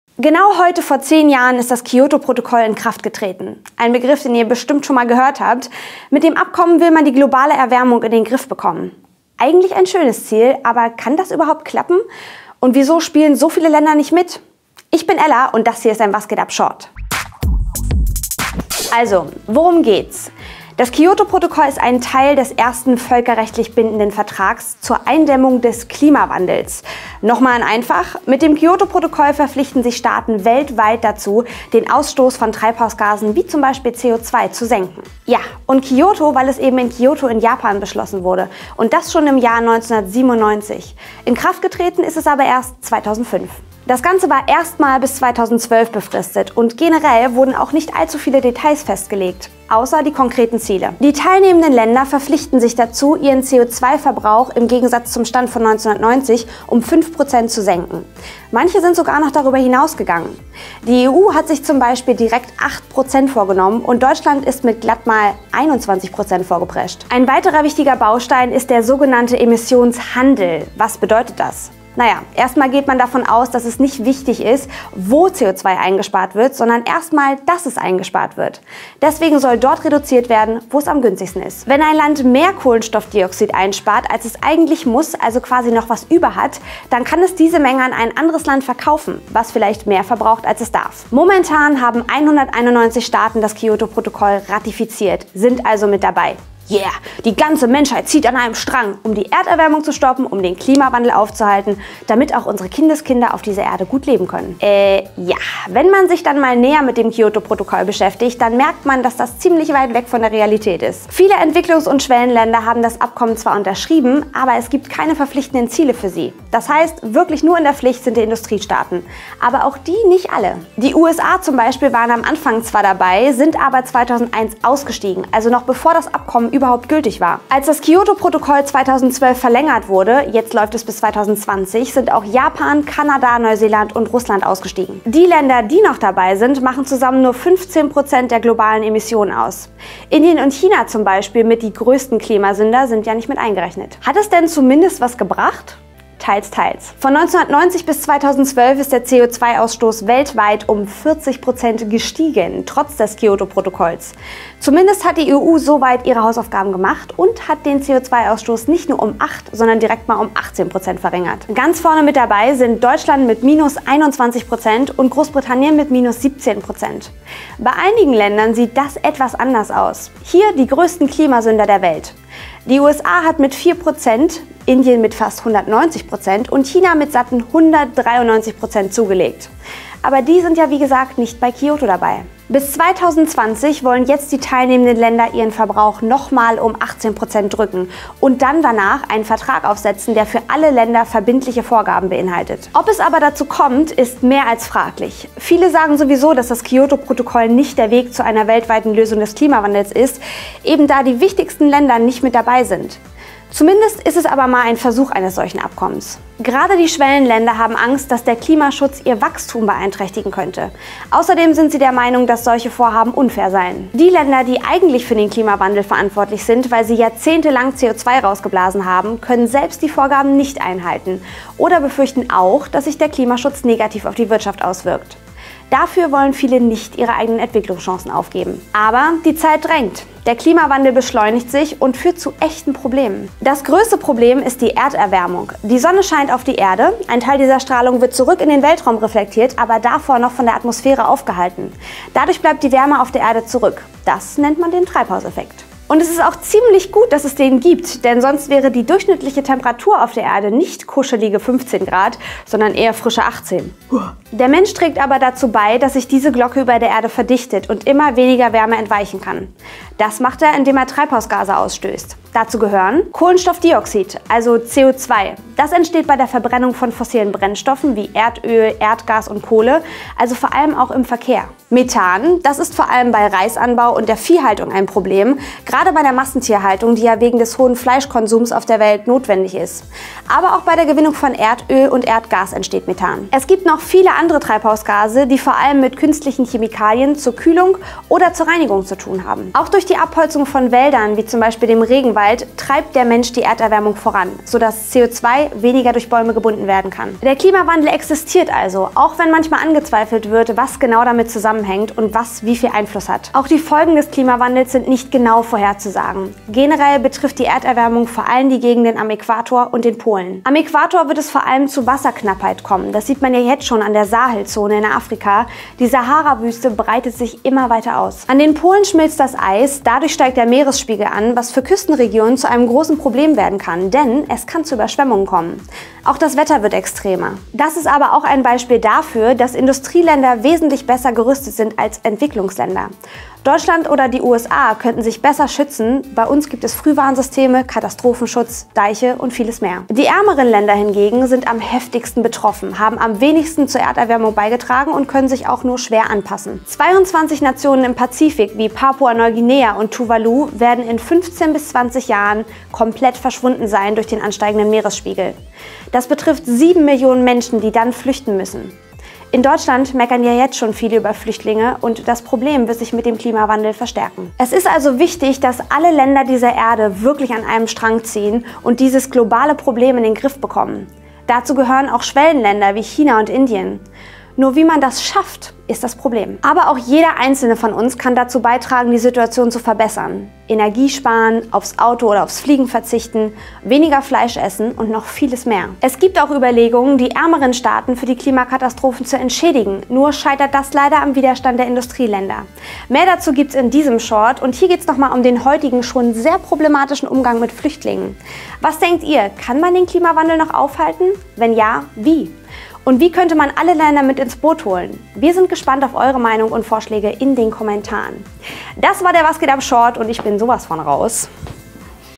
Klima-Fail - Zu spät für Rettung?
Heute vor 10 Jahren ist das Kyoto-Protokoll in Kraft getreten - das weltweit erste Abkommen zum Klimaschutz. Eine gute Sache - denkt man! Gebracht hat’s nicht so viel. Wieso das so ist, und warum das wichtig ist erfahrt ihr hier!

Moderator: Ella
Redaktion: Arne Fleckenstein
Regie: Daniel Czepelczauer
Kamera: Julian Zahn
Ton: Julian Zahn
Schnitt: Mike

Visuelle Konzeption: Astrid Deinhard-Olsson
Executive Producer: Marius Stolz
Head of Production: Michael Wendt
Social Media Manager: Laura Pagan